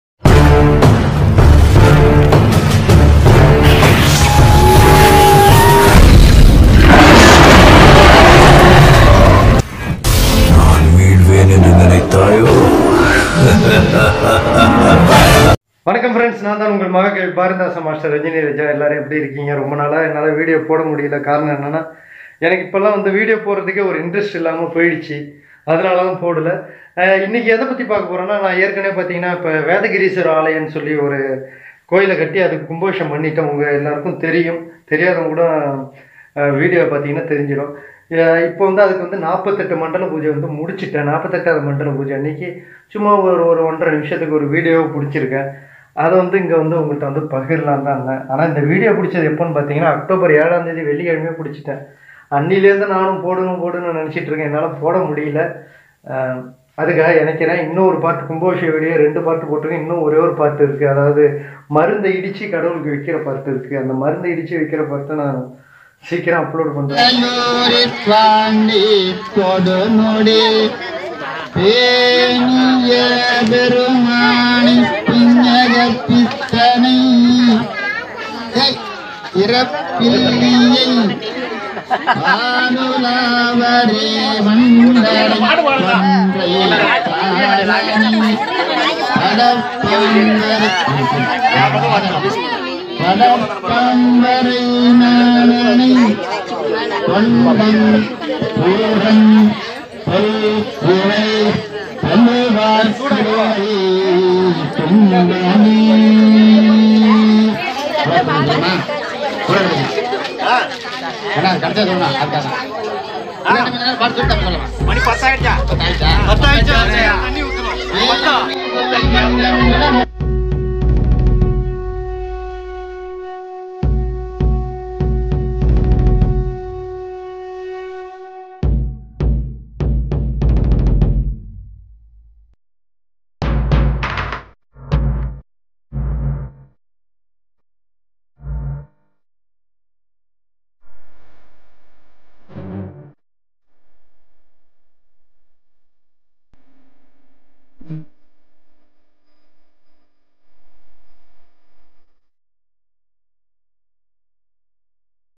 ஸ்ரீவேதகிரீஸ்வரர் ஆலய மண்டல பூஜை நிறைவு விழா காணொளி அக்டோபர் 7 2022/சிவாச்சாரியார்/மேலணிக்குழி
கார்த்திக்கேயன் MRV ராஜா மேல் நிலைப்பள்ளி பாப்பாக்குடி தலமை ஆசிரியர் மற்றும் நமச்சிவாயம் என்கிற சிவ சிவ இரு சிவாச்சாரியார்களை வைத்து அக்டோபர் 7 வெள்ளிக்கிழமை ஸ்ரீவேதகிரீஸ்வரர் ஆலயத்திற்கு மண்டல பூஜை வைத்தோம்.ஒரு ரூபாய் கூட பெறாமல் சேவை செய்தனர்

Keep Your Support to My Channel.